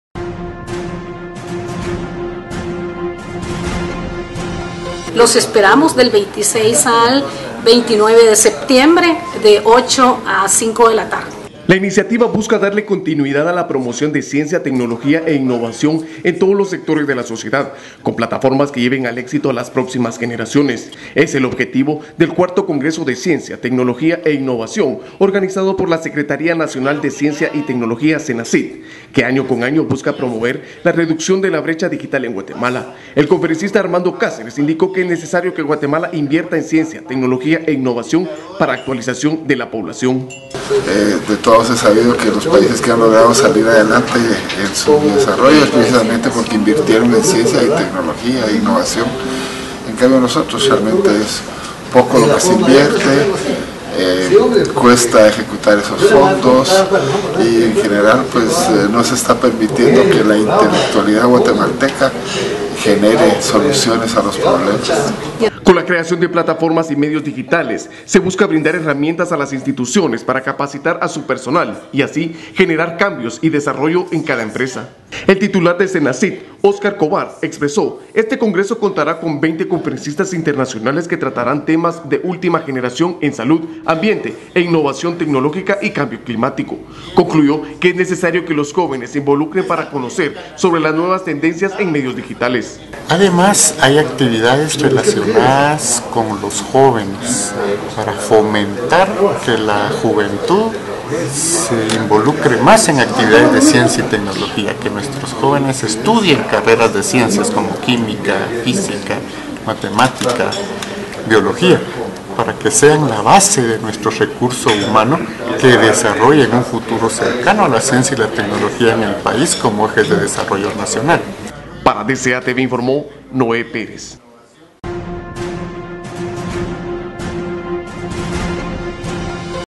Buscan que jóvenes se interesen por ciencia, tecnología e innovación
La iniciativa busca darle continuidad a la promoción de ciencia, tecnología e innovación en todos los sectores de la sociedad, con plataformas que lleven al éxito a las próximas generaciones. Es el objetivo del IV Congreso de Ciencia, Tecnología e Innovación organizado por la Secretaria Nacional de Ciencia y Tecnología SENACYT, que año con año busca promover la reducción de la brecha digital en Guatemala. El conferencista Armando Cáceres,  indicó que es necesario que Guatemala invierta en ciencia, tecnología e innovación para la actualización de la población.

Con la creación de plataformas y medios digitales se busca brindar herramientas a las instituciones para capacitar a su personal y así,  generar cambios y desarrollo en cada empresa.

El titular de la SENACYT, Oscar Cóbar expresó, este congreso contará con 20 conferencista internacionales que trataran temas de última generación en salud, ambiente innovación tecnología y cambio climático.  Concluyó que es necesario que los jóvenes se involucren para conocer sobre las nuevas tendencia de los medios digitales.